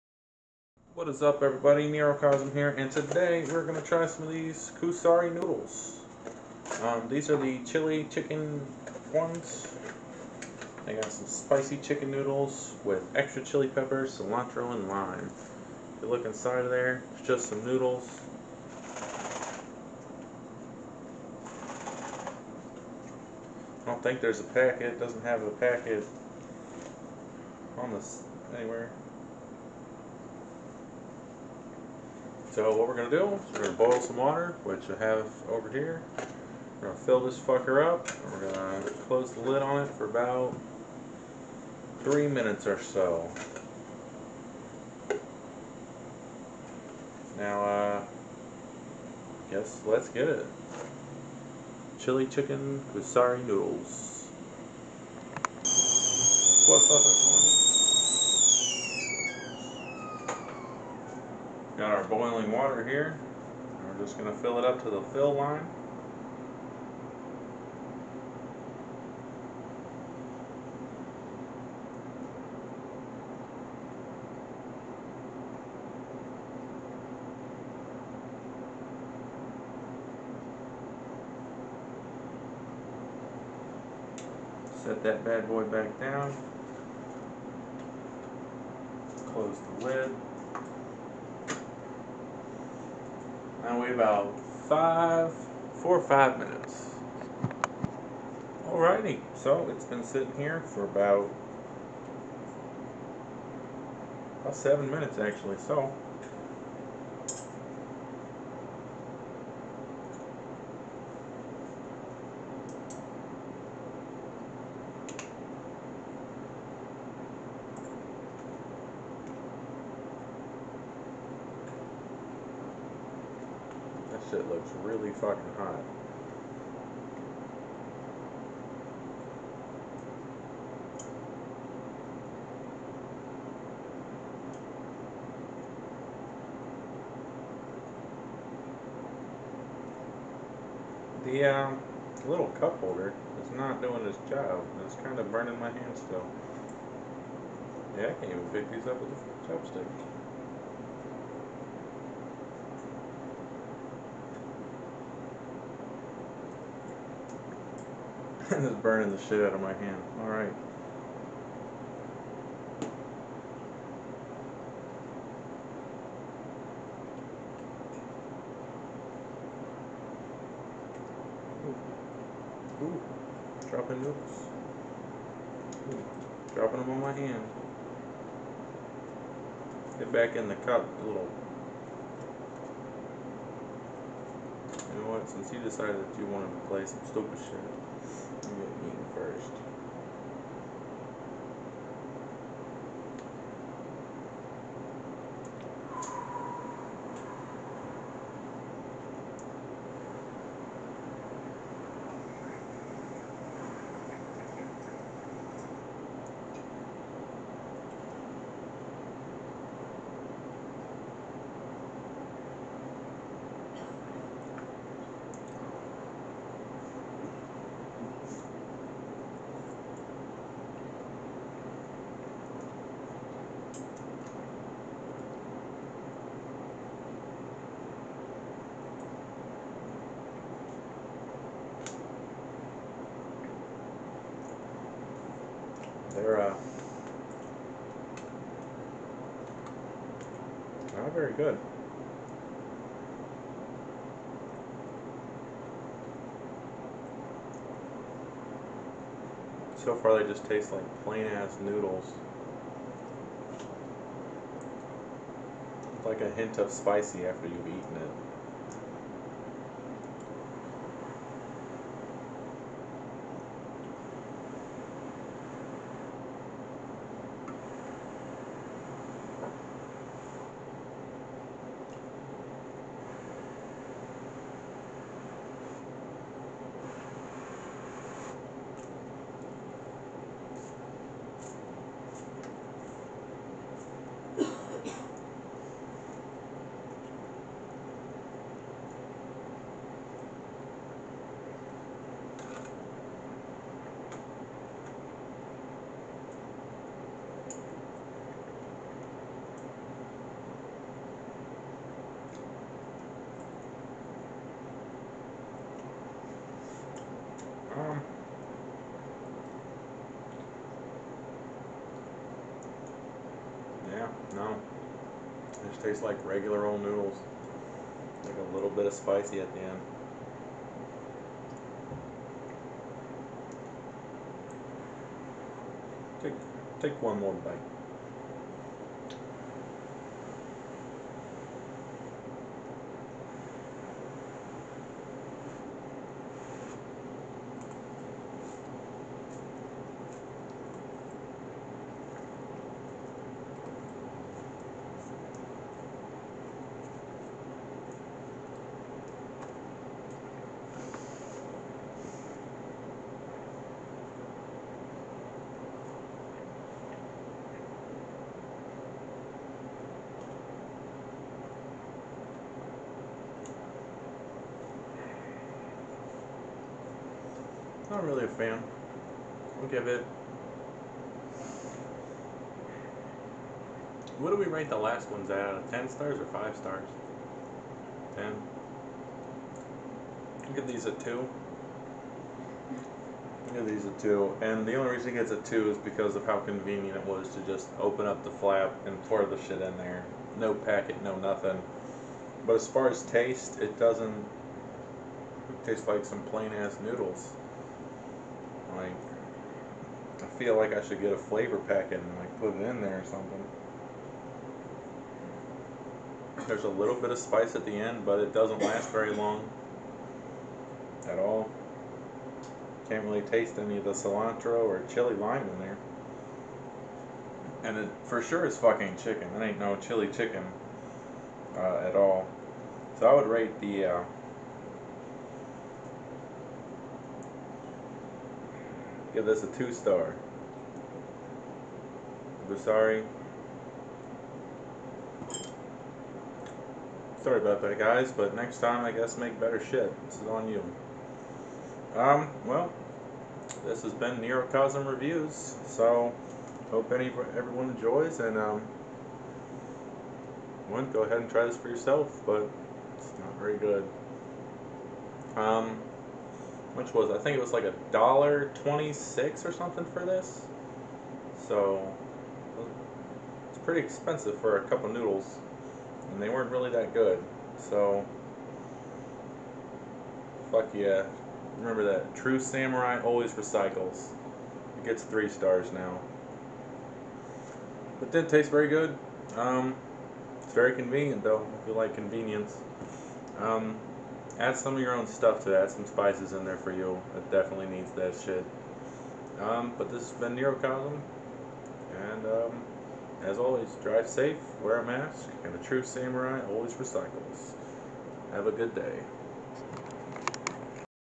Kusari Noodles Review
Nerocosm tries chili chicken flavored Kusari noodles.